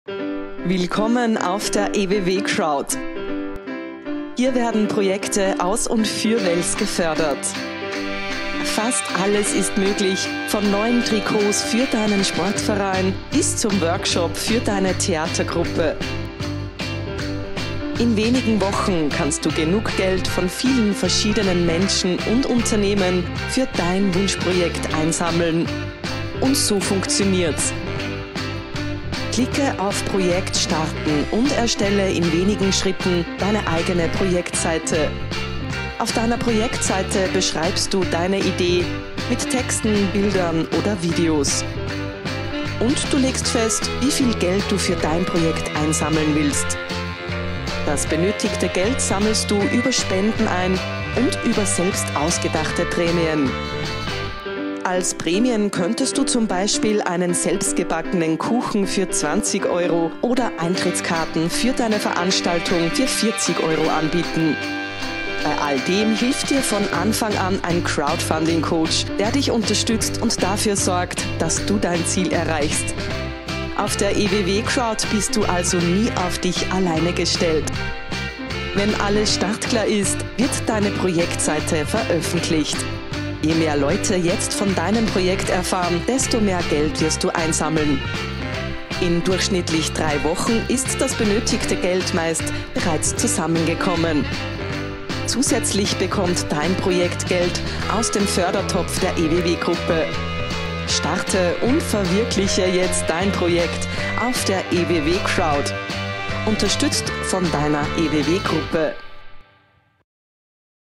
eww Crowd | Erklärvideo zur Crowdfunding-Plattform
Mit unserer Crowdfunding-Plattform verleihen wir lokalen Projekten Flügel. Ein Projekt auf der eww Crowd ist eine Crowdfunding-Kampagne, bei der du selbst oder mit deinem Verein, Organisation, Unternehmen, etc. Geld für verschiedenste Vorhaben sammeln kannst.

Wir helfen bei der erfolgreichen Suche nach Unterstützer:innen, die dich bei deinem Vorhaben finanziell unterstützen. Diese können innerhalb eines festgelegten Zeitraums mit einem freien Betrag spenden und/oder sich eine von dir oder von unseren Partnern angebotene Prämie auswählen.

Unser Crowdfunding-Team unterstützt dich dabei. Los geht's